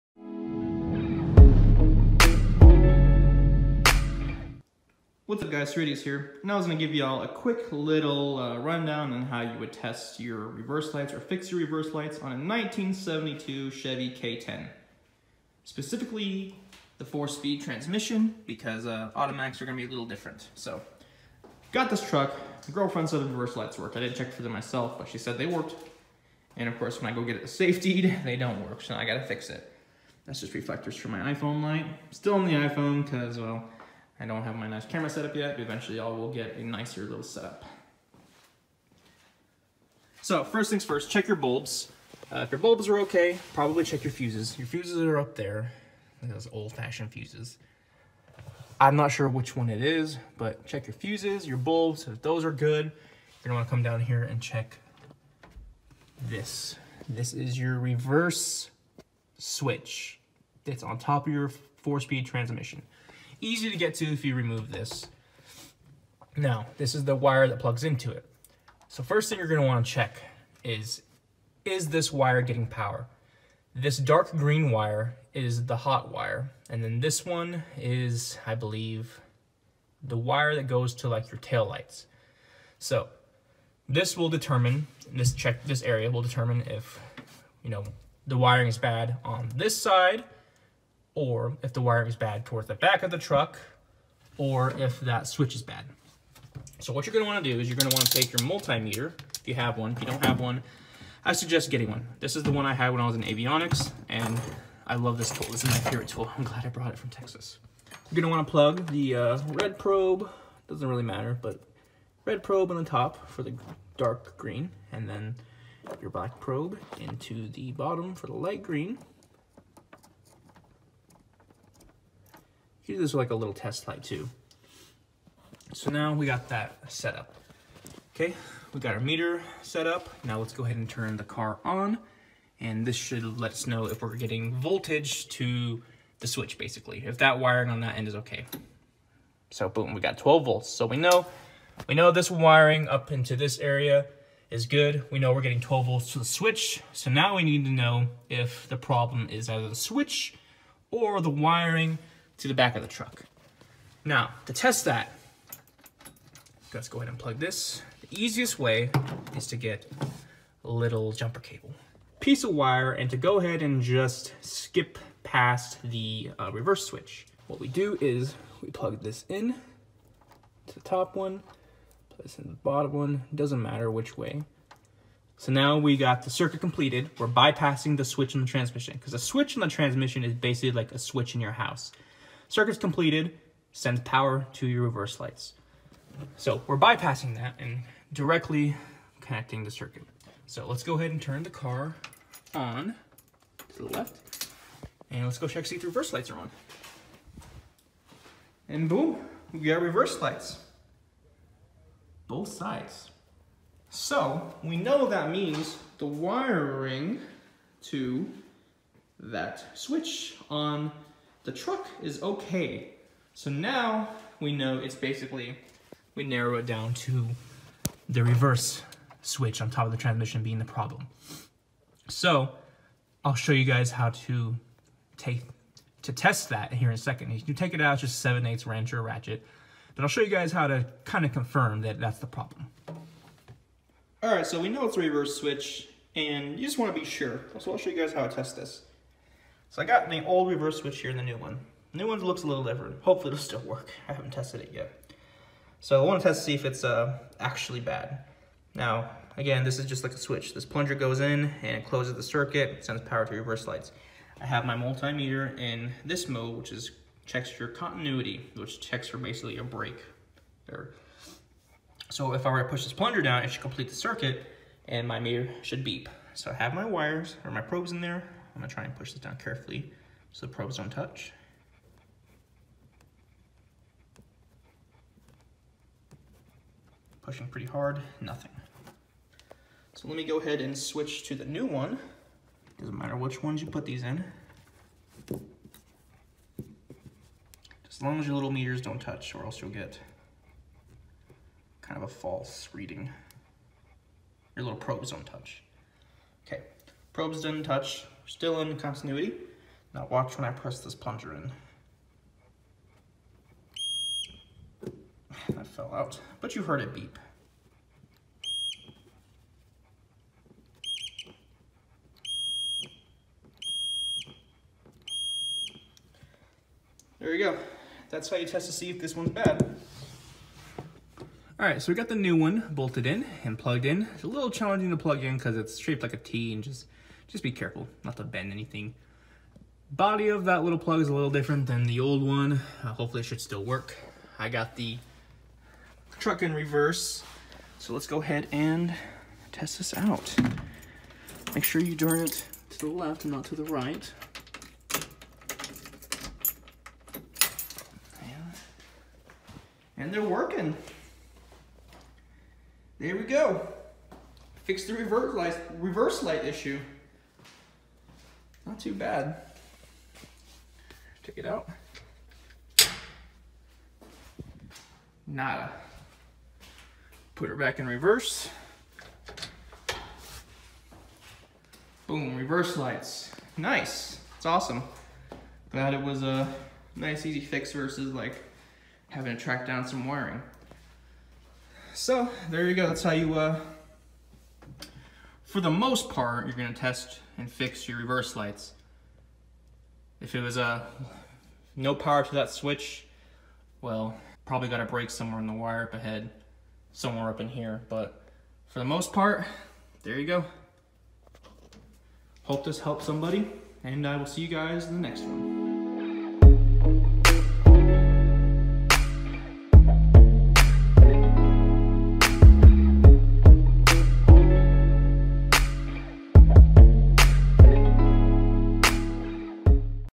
67 72 C K10 4speed Reverse Lights Fix
Reverse switch can be ordered at LMCTruck.com The partnumber is  36-0660